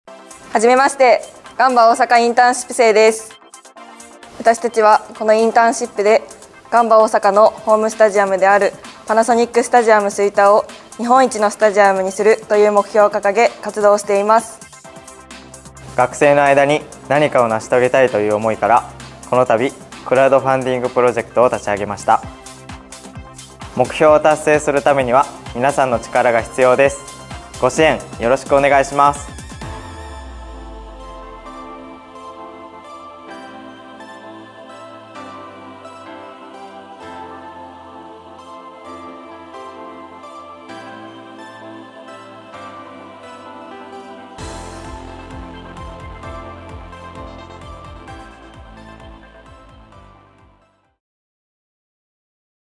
ガンバ大阪インターン生よりクラウドファンディングご協力のお願い（追手門学院大学）
追手門学院大学「ガンバ大阪インターンシップ」は、学生の力で「パナソニックスタジアム吹田を日本一に！」を目標に、スタジアム運営などの活動をしています。
このたび皆様からご支援をいただきたく、クラウドファンディングを開始しました。
まずはインターン生からの挨拶を動画でお送りします。ぜひご覧ください。